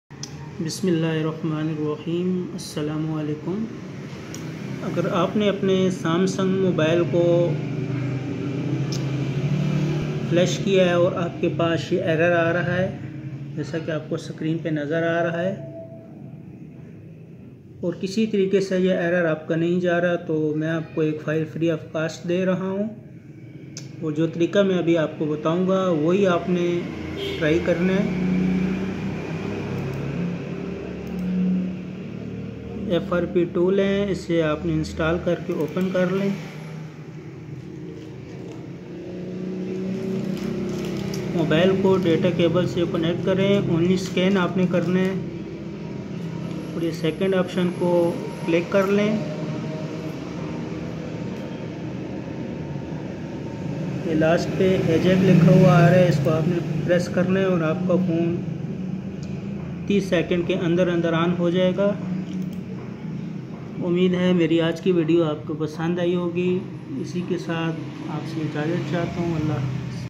Firmware upgrade encountered an issu please select recovery mode in kies & try again AttockSharif
issu resolve v.easy free
tool link is.

password is.
no passsword required